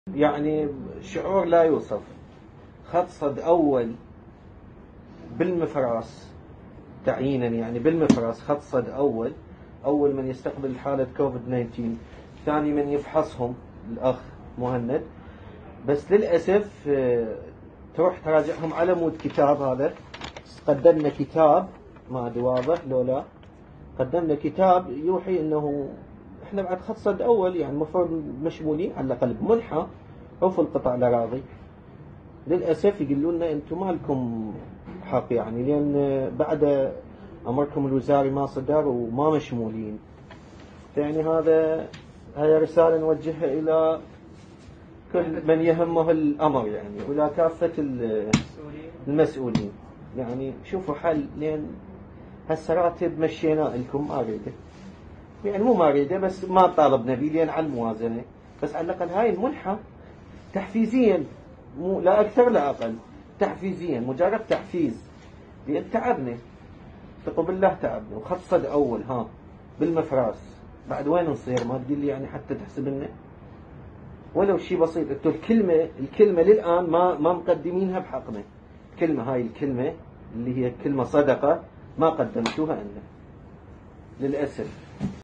العاملون بصفة "أجر يومي" في القطاع الصحي يطالبون الحكومة بصرف مستحقاتهم المالية ومراعاة ظروفهم
العاملون بصفة "أجر يومي" في القطاع الصحي يطالبون الحكومة بصرف مستحقاتهم المالية ومراعاة ظروفهم كونهم يعملون في خط الصد الأول مع المصابين بفيروس
ــــــــــــــــــــــــــــ